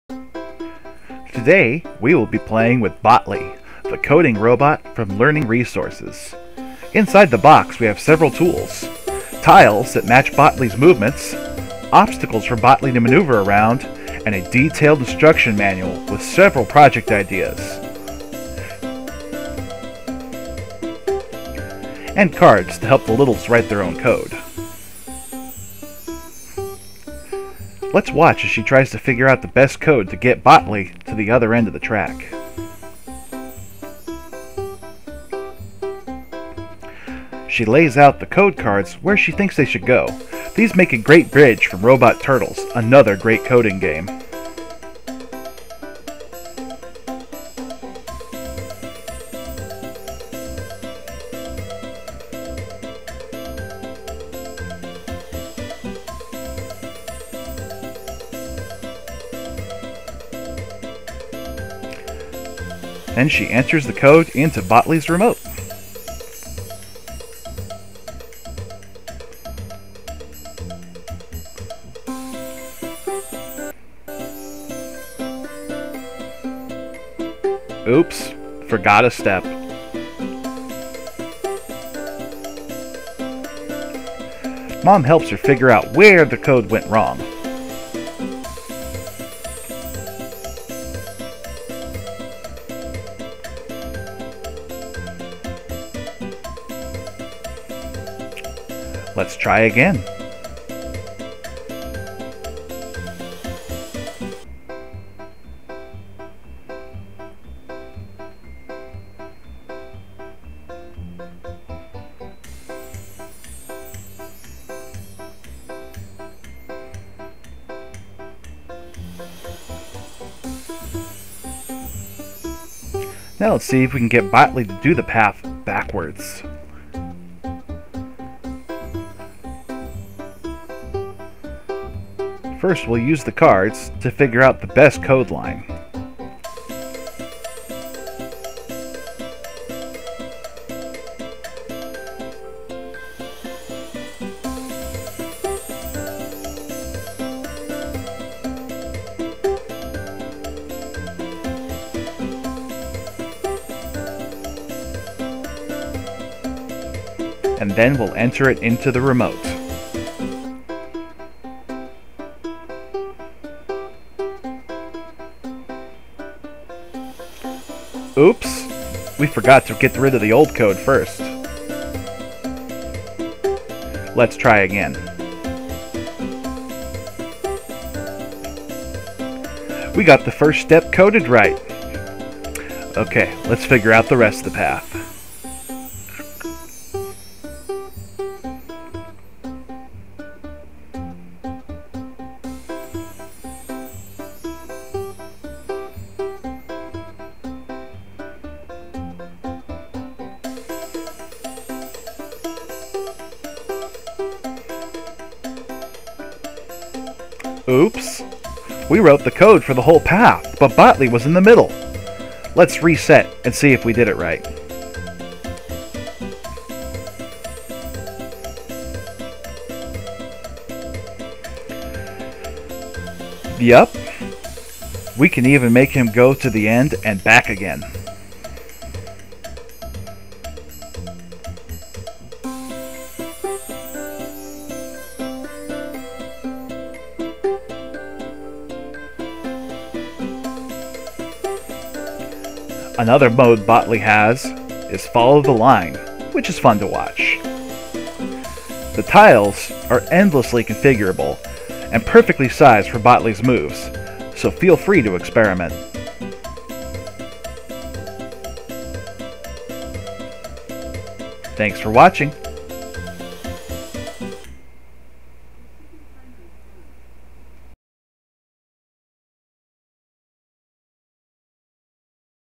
Botley Coding Robot
Watch my daughter play with our Botley Coding Robot, sharpening her coding skills.

I'd appreciate a click on my paid link (no cost to you)